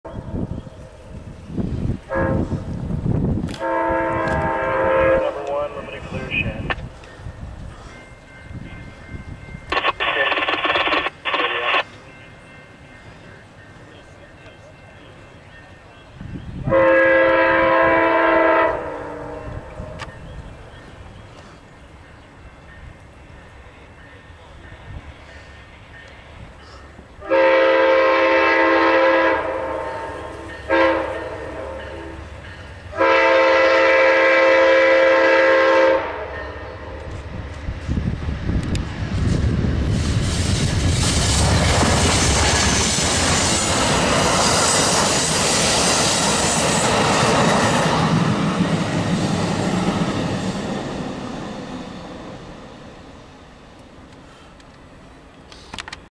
#15971 Amtrak P030
Amtrak P030 is heading eastbound from track 1-2